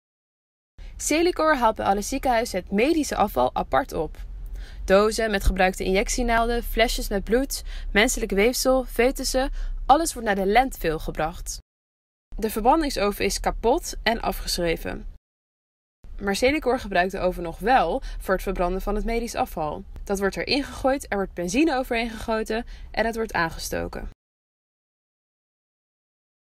Medisch afval zoals foetussen gaat in kapotte verbrandingsoven
Selikor, het afvalverwerkingsbedrijf van de overheid, haalt elke week ziekenhuisafval op. Daar zitten menselijke resten en bebloede operatiemiddelen in die in de verbrandingsoven bij Malpais worden verbrand. Die oven is echter afgeschreven.